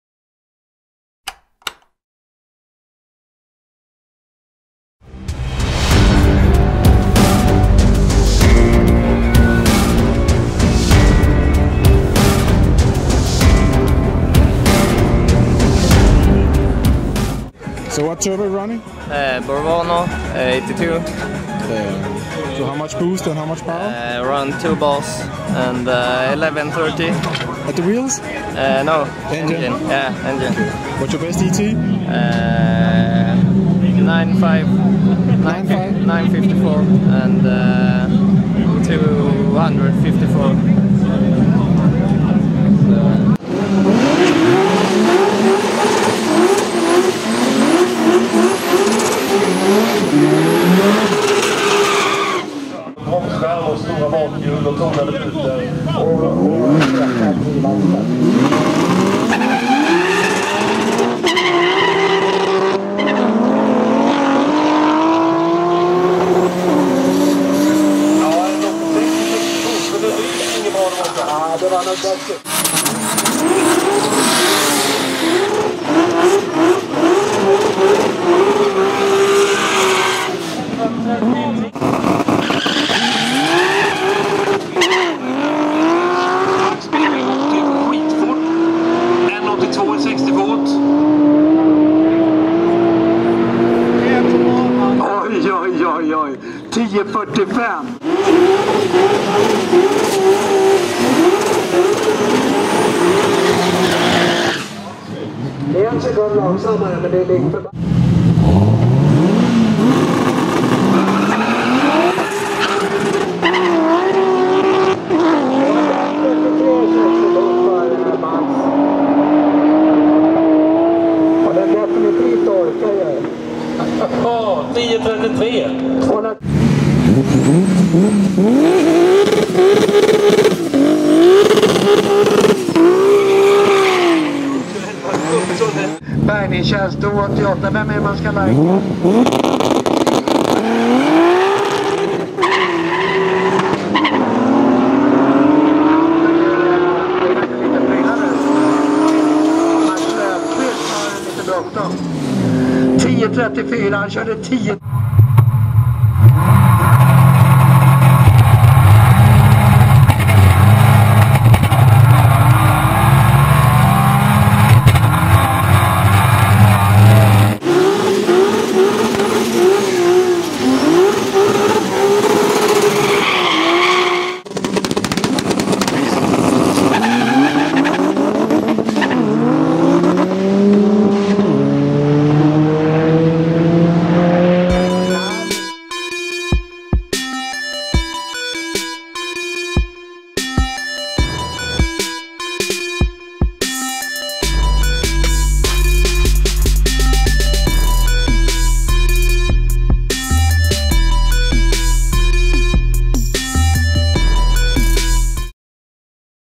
1100 Hp ! E30 Racing at EDPS 2017
This E30 is pretty badasswith 1100 HP on tap powered with an S50b30 3,3 L and a Bw 82 mm turbo on e85.
this car has been in the 9.50s but not today.
what everybody whant to know can this car beat a 1200Hp gtr on the highway ?

for all danish car related stuff !